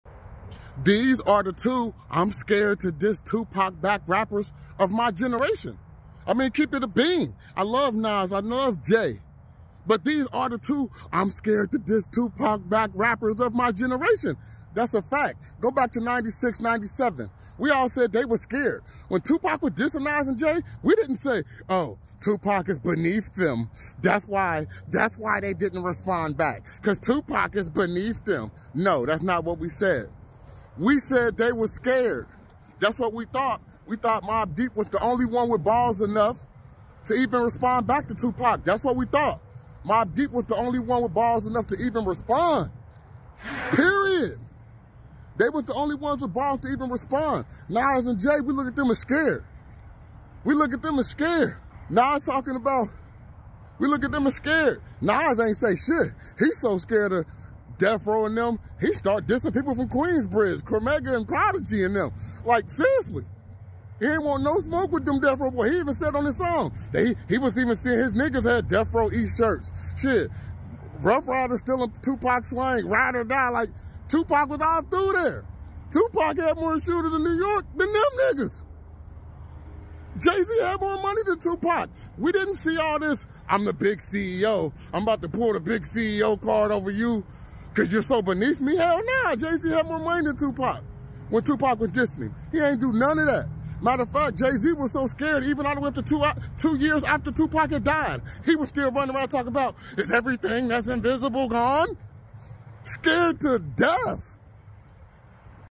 the two I'm scared to diss Tupac back rappers of my generation
jayz and Nas were scared to respond to Tupac before his death